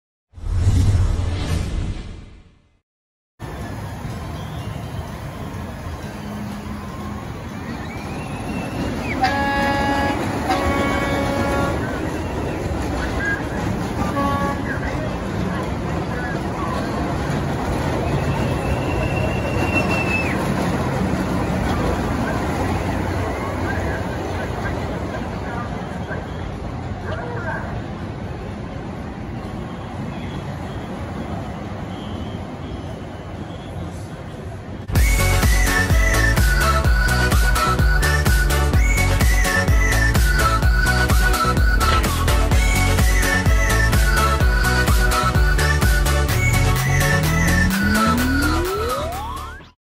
Steel Lasso Off Ride Frontier City July 2020
In this video, I showcase off ride footage I caught of Frontier City’s Steel Lasso Roller Coaster.

Footage filmed by Coliwood Studios and Chris.

Featuring the Coliwood Studios Lone Star Summer Trip Shirt, and the Coliwood Studios America Logo Shirt!